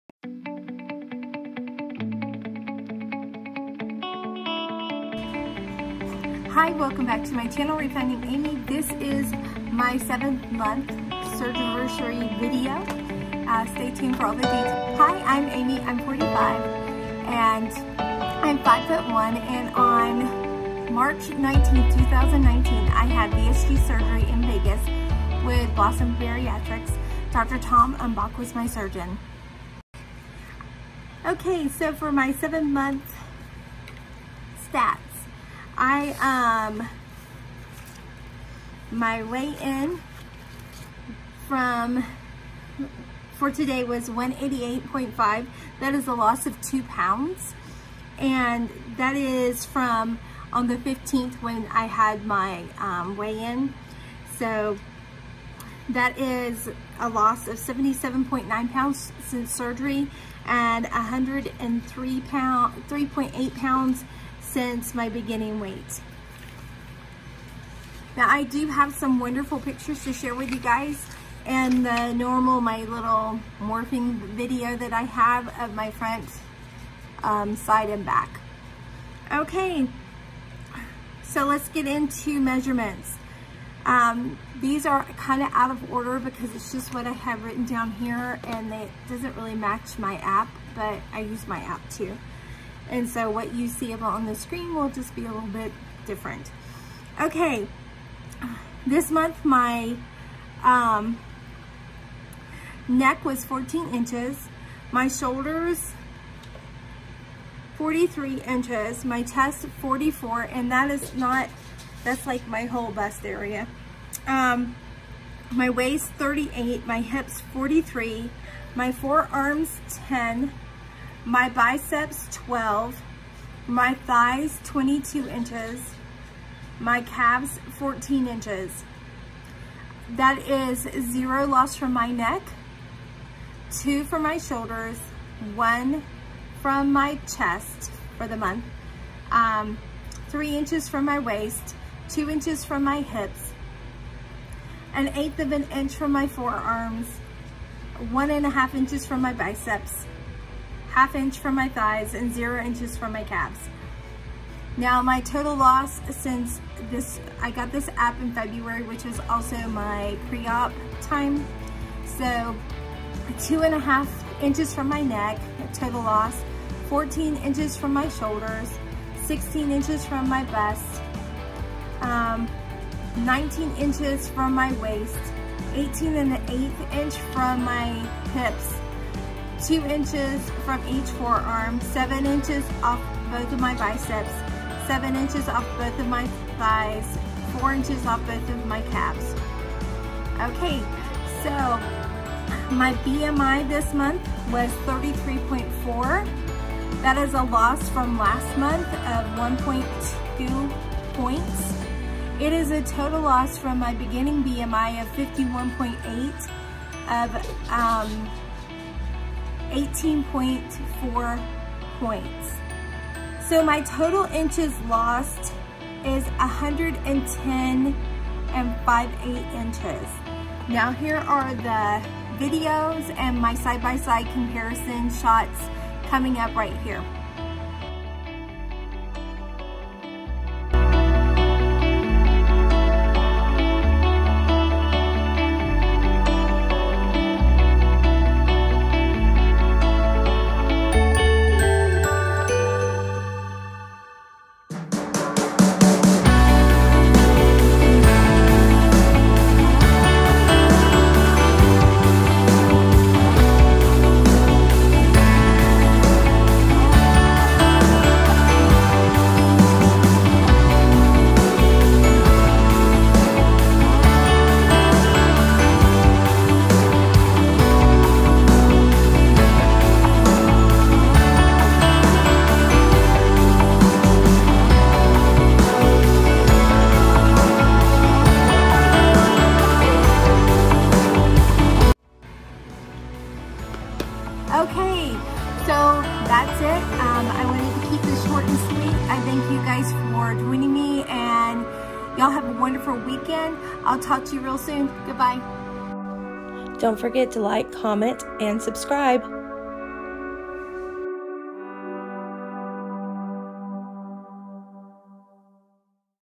7 month Surgiversary
My stats
Highest Weight: 336
Beginning Weight: 292.3
Preop Weight: 285 (corrected from 280)
Surgery Weight: 266.4
Current Weight: 188.5
Total Weight Loss: 103.8